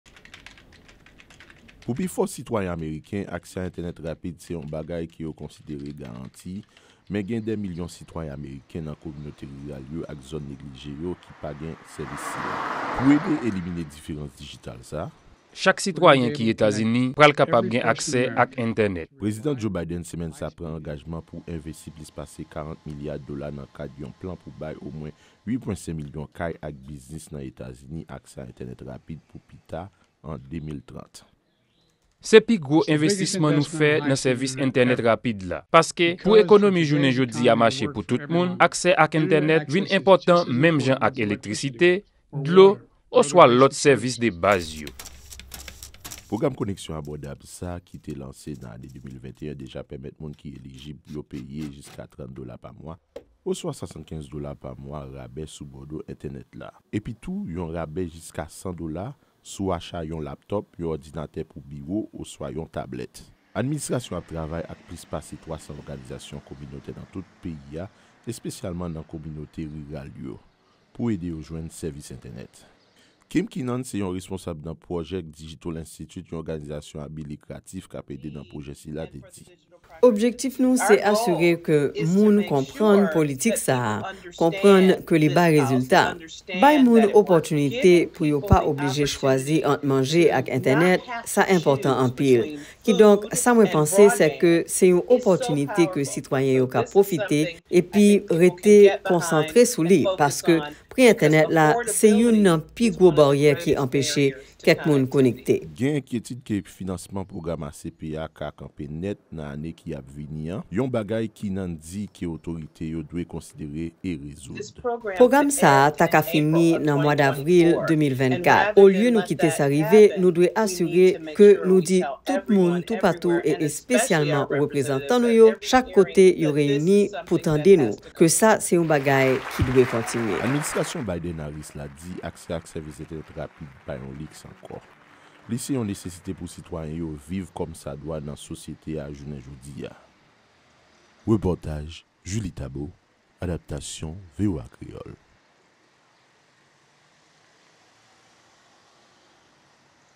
Administrasyon Biden Nan vle Bay Tout Sitwayen Ameriken Aksè ak Entènèt
Prezidan Etazini Joe Biden di li vle asire li ke chak sitwayen ameriken gen aksè ak entènèt. Etazini pral envesti plis pase $40 milya dola pou elaji sèvis intenet li a .  Repòtaj Jounalis VOA News Julie Taboh